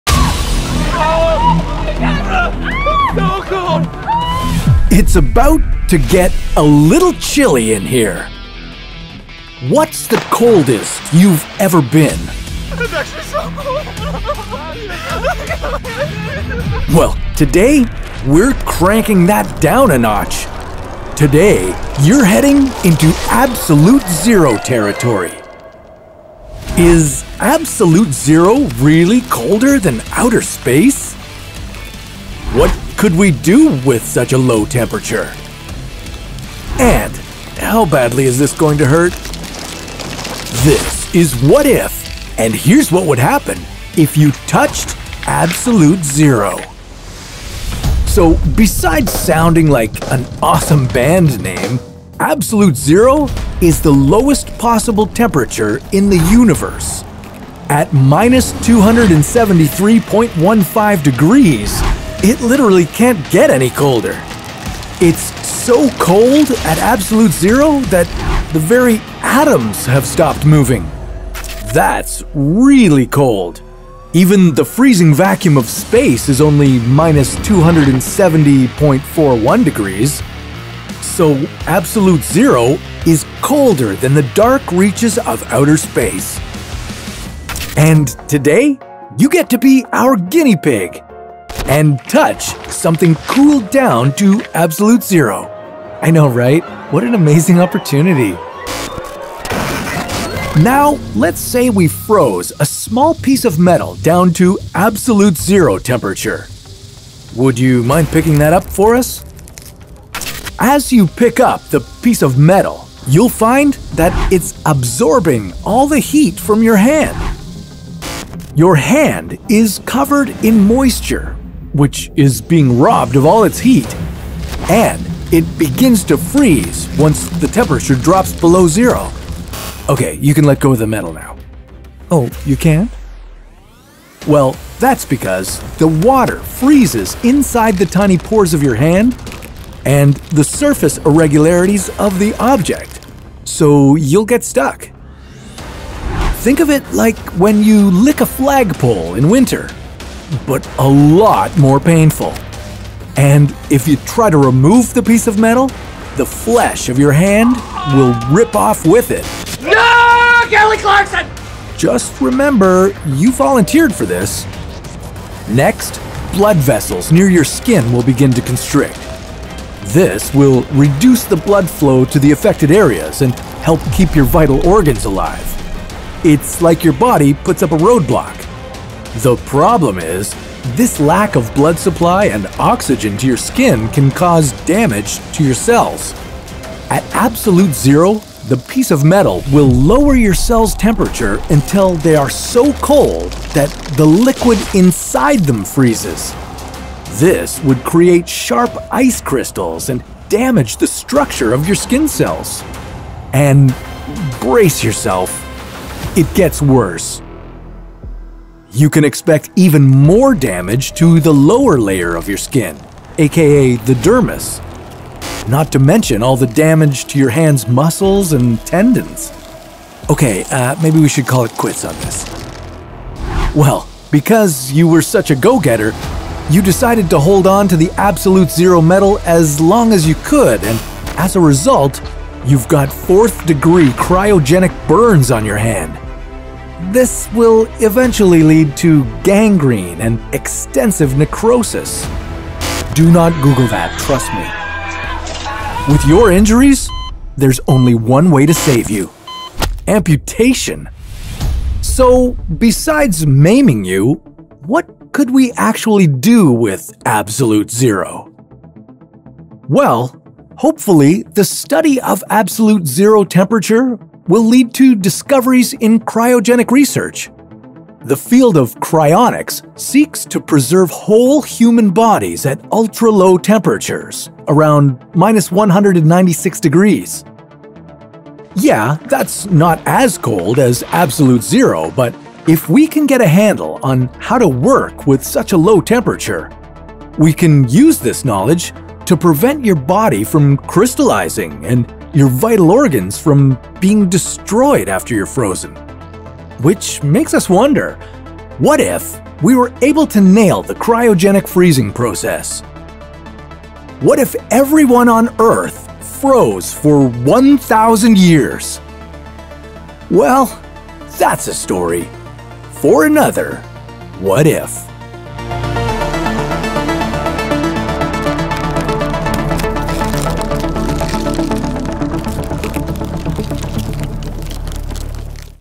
What If You Touched Absolute Zero?
Well today, we’re cranking that down a notch. Today, you’re heading into absolute zero territory. Is absolute zero really colder than outer space? What could we do with such a low temperature? And how badly is this going to hurt?

Thank you to our supporters:
Batman
Kristopher Kirby